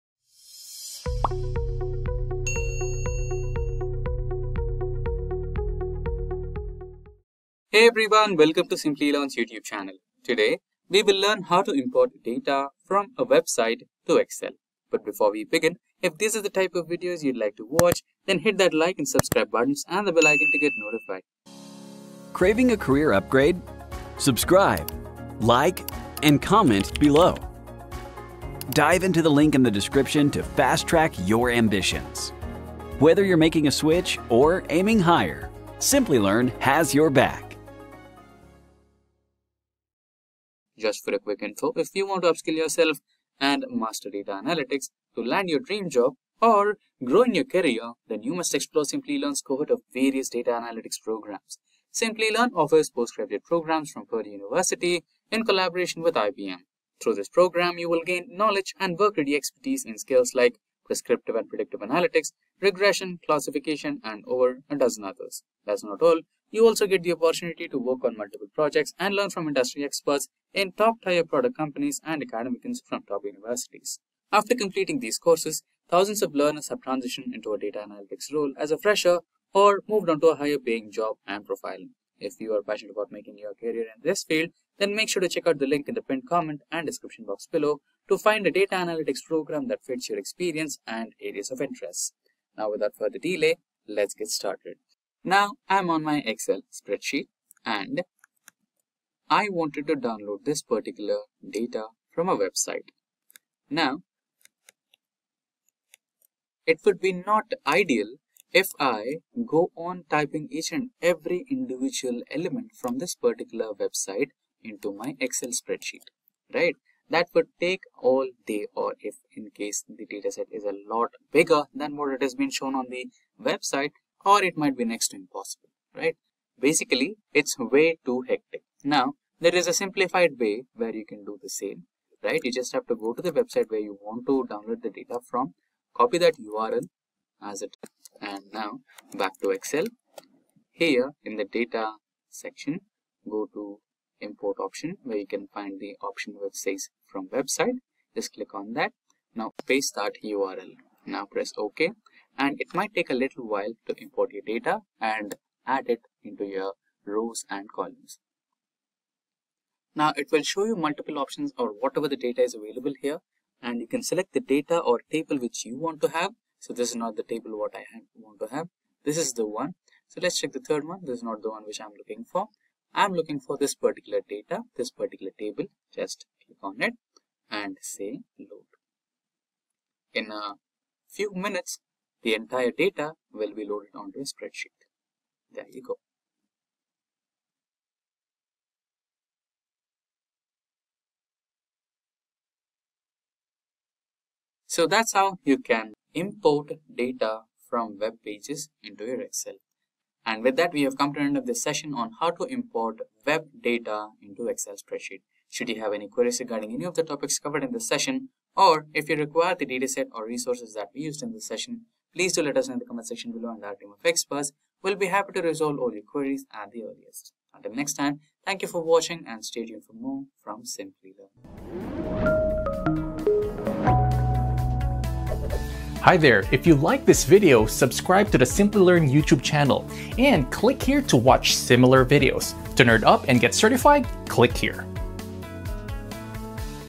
How to Import Data from Web to Excel | Import Live Data From Web to Excel | Web Query | Simplilearn
️️🔥 Professional Certificate in Data Analytics and Generative AI, by Simplilearn in collaboration with Purdue University

This video by simplilearn is dedicated to help aspiring data analysts and business analysts learning how to get connected to real time data on a website to excel and import the data from the website. Welcome to our latest tutorial where we dive into the world of data manipulation! In this step-by-step guide, we'll show you the ropes on importing data from the web directly into Microsoft Excel, unleashing a wealth of possibilities for your analytical endeavors.

🔍 What You'll Learn:

Introduction to Web Data Importation
Navigating Excel's Web Query Feature
Hands-On Demonstration with Real-World Examples
Tips and Tricks for Streamlining the Process

✅ Skills Covered
- Data Analytics
- Supervised Learning
- Unsupervised Learning